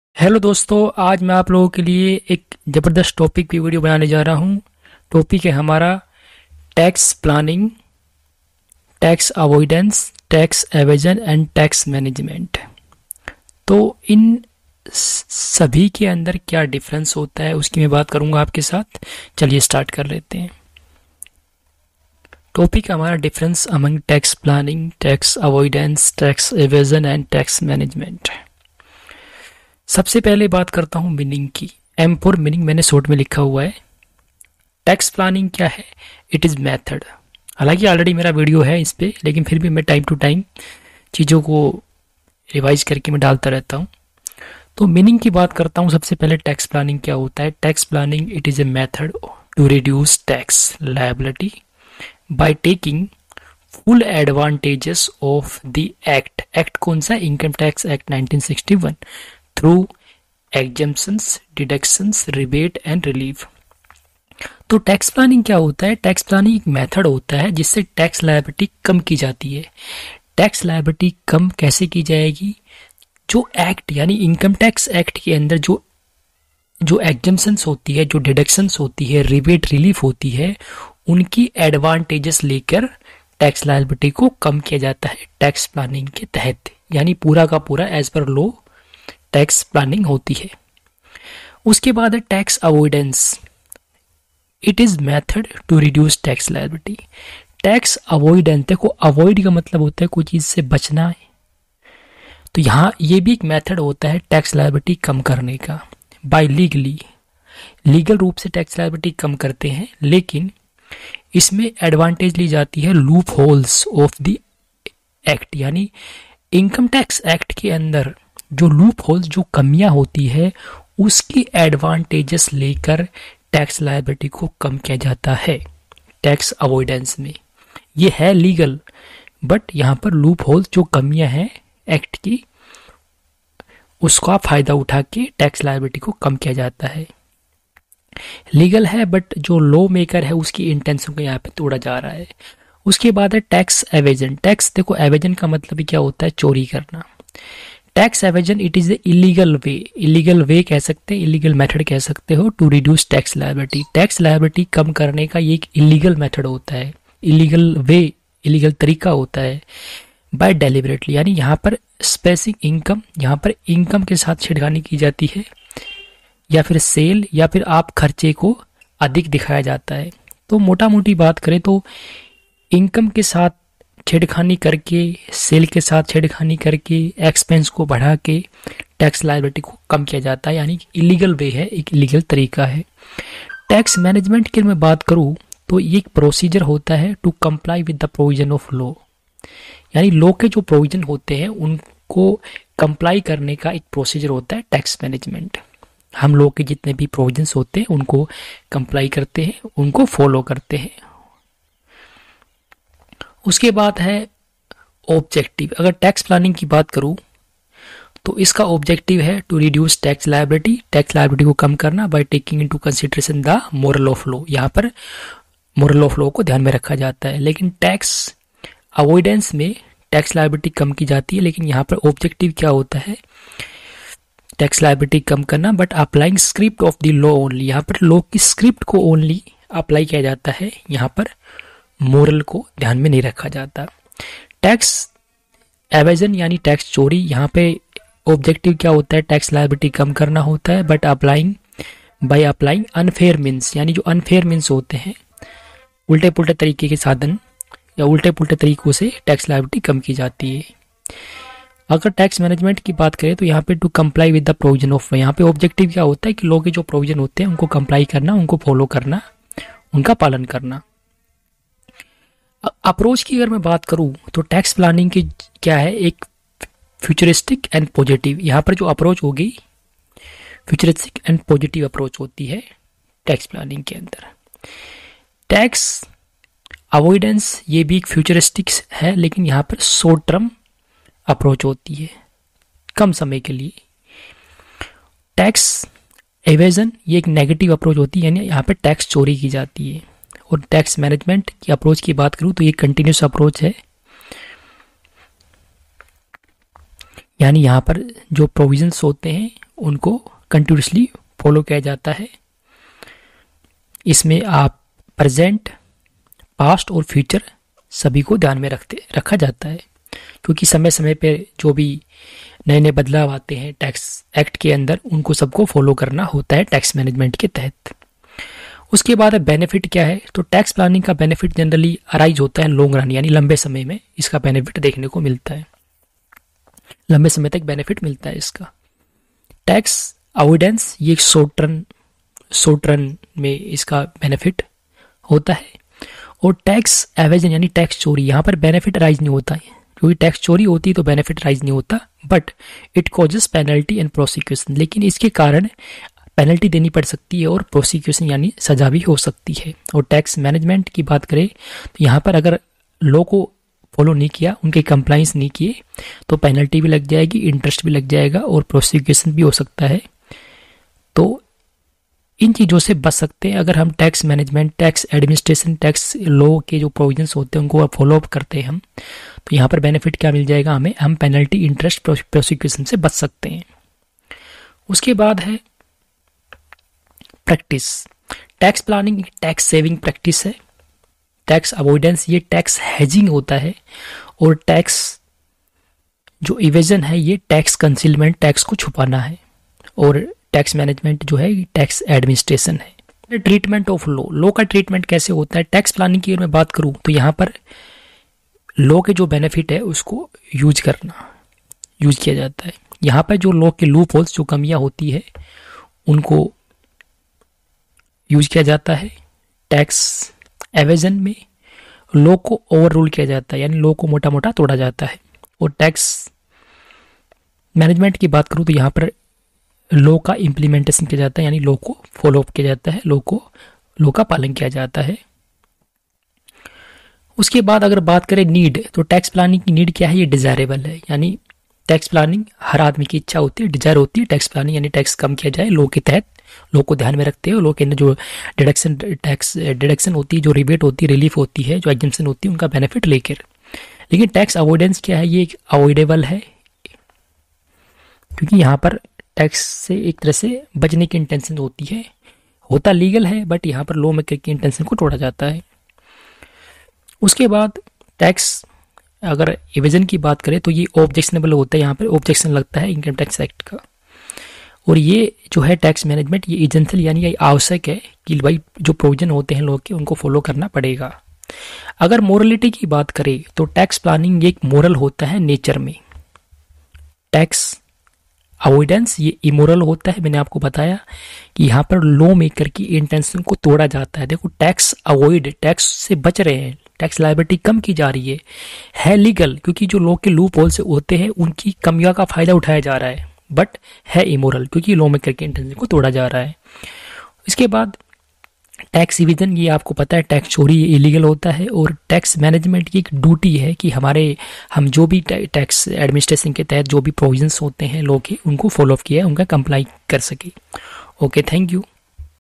Tax Planning, Tax Avoidance, Tax Evasion & Tax Management
Difference among tax planning, tax avoidance, tax evasion and tax management
l